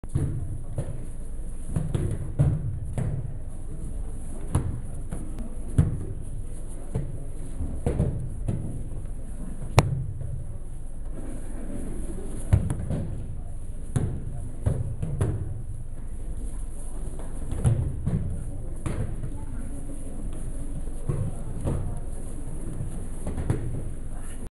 Aikidoka Kurt from Belgium...
Aikidoka Kurt train on New Year's internship Flemish VAV Federation of Belgium in dojo De Bres in Halle Belgium on Saturday, January 12, 2013 ... :-)